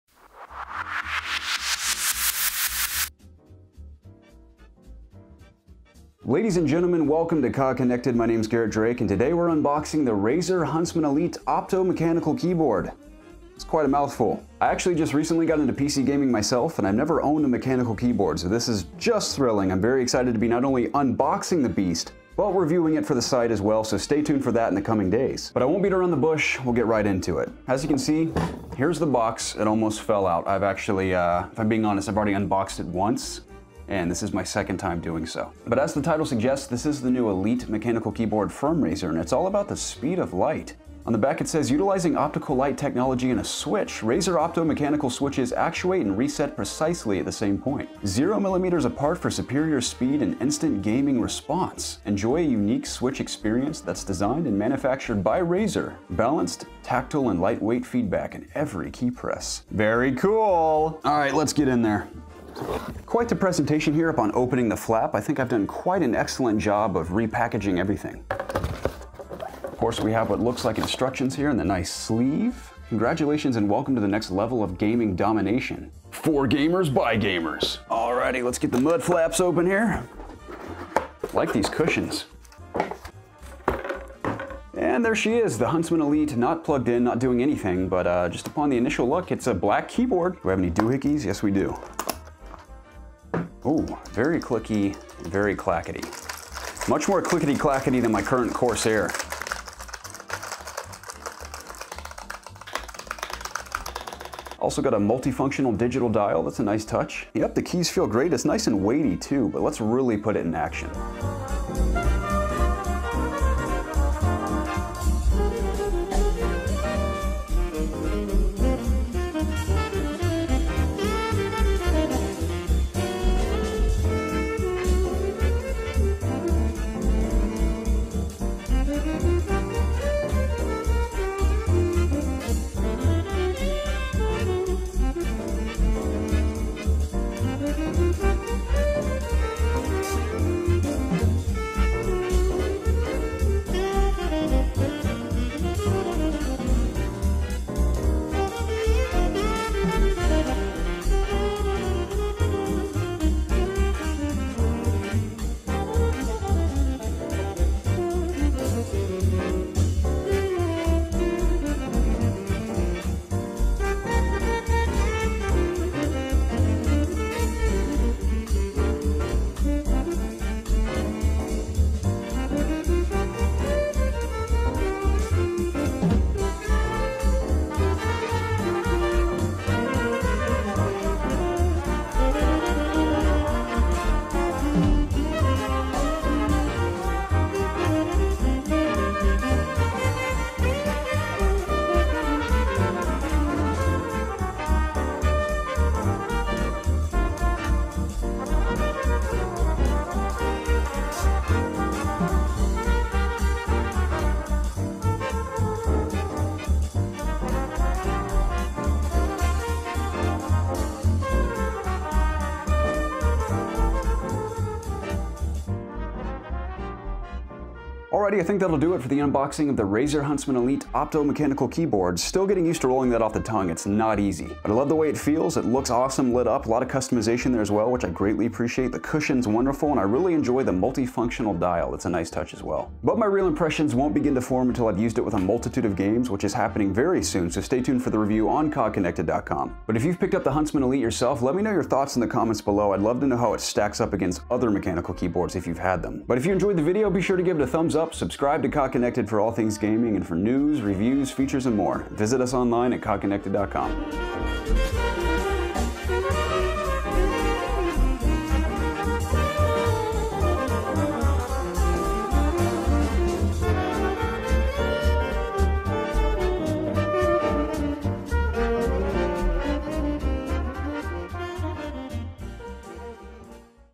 Razer Huntsman Elite Opto-Mechanical Gaming Keyboard Unboxing
We unbox Razer's new Huntsman Elite Opto-Mechanical Gaming Keyboard!